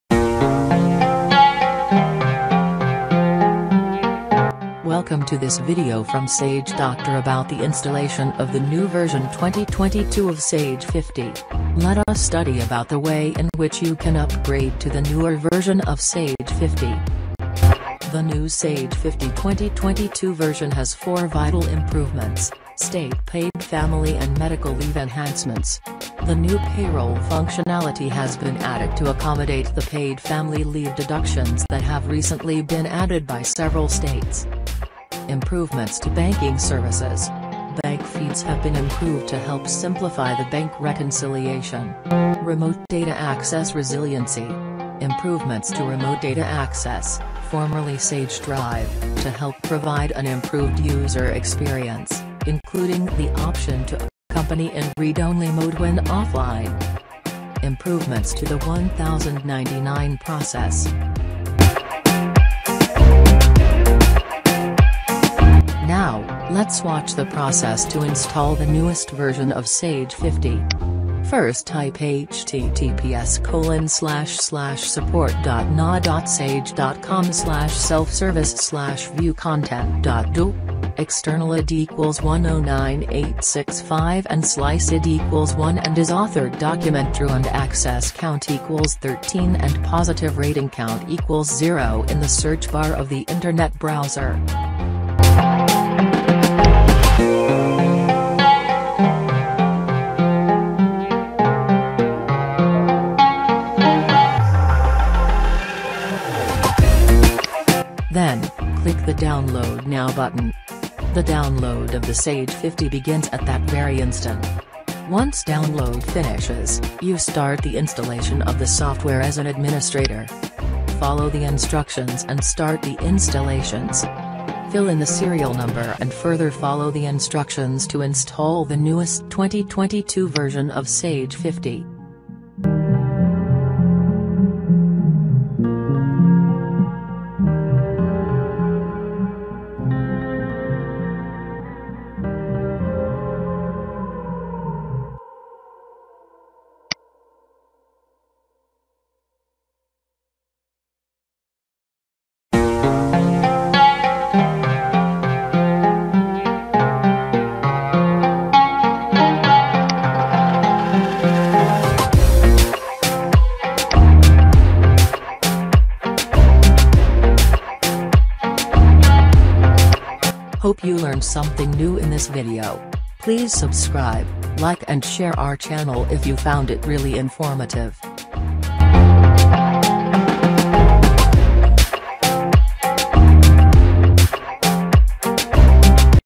Sage50 2022 Version | Download and Install | New Features
Schedule a Meeting with Us The training is designed specifically for the users who want to use Sage 50 as an accounting tool for their company.

We at Sage Doctor can help install the new software version along with technical assistance to make the transition from the earlier versions to 2022 version.

The list of improvements is short, but they will be significant to some users.

State Paid Family and Medical Leave enhancements
New payroll functionality has been added to accommodate the paid family leave deductions that have recently been added by several states.

Improvements to Banking Services
Bank feeds have improved to help simplify bank reconciliation.

Remote Data Access resiliency
Improvements to Remote Data Access (formerly Sage Drive) to help provide an improved user experience, including the option to open a company in read-only mode when offline.

Improvements to the 1099 process.